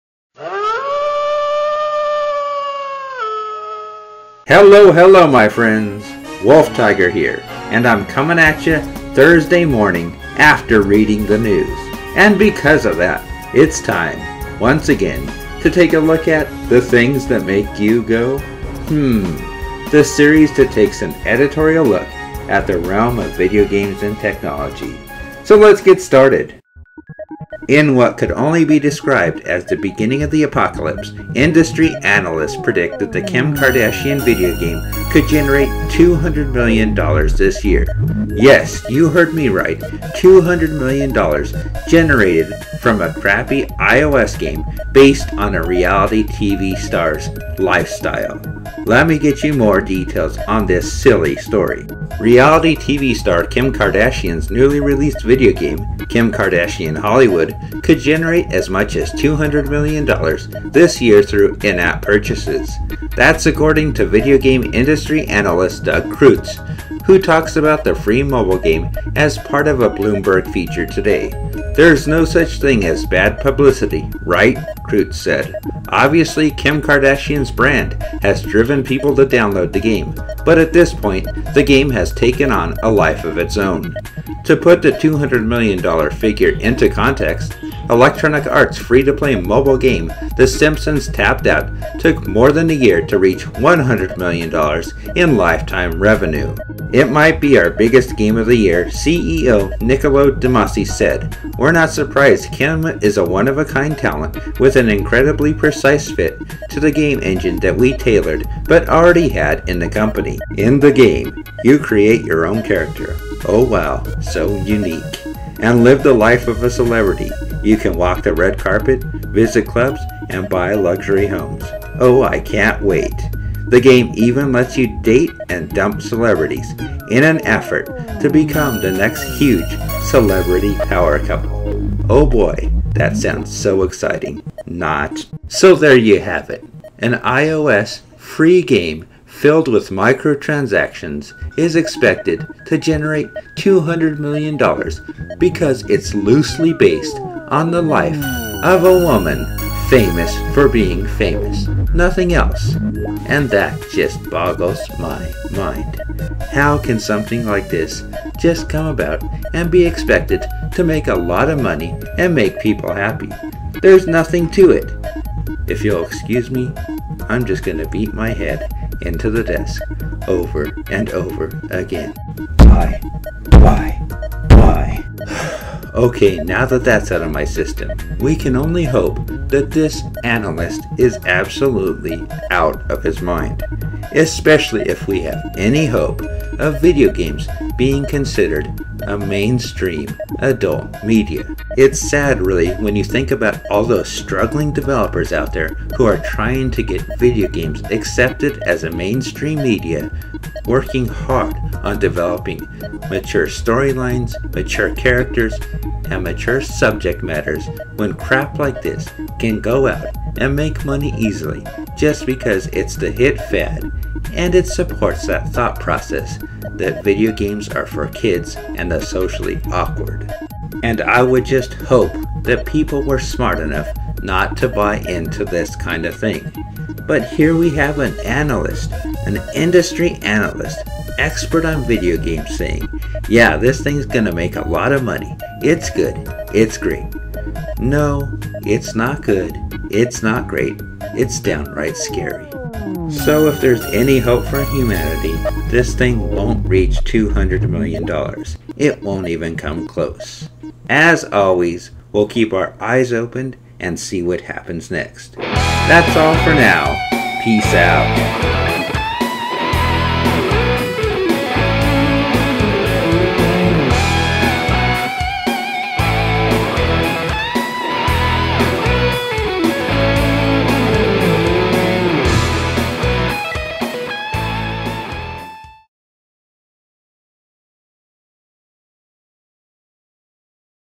Industry Analyst Predicts Kim Kardashian's IOS Game Could Make $200 Million This Year
Reality TV star Kim Kardashian's newly released video game, Kim Kardashian: Hollywood, could generate as much as $200 million this year through in-app purchases. That's according to video game industry analyst Doug Creutz, who talks about the free mobile game as part of a Bloomberg feature today.

"There's no such thing as bad publicity, right?" Creutz said. "Obviously, Kim Kardashian's brand has driven people to download the game. But at this point, the game has taken on a life of its own."

Things that make you go Hmm is an editorial and articles series that covers the video game industry and video game related topics.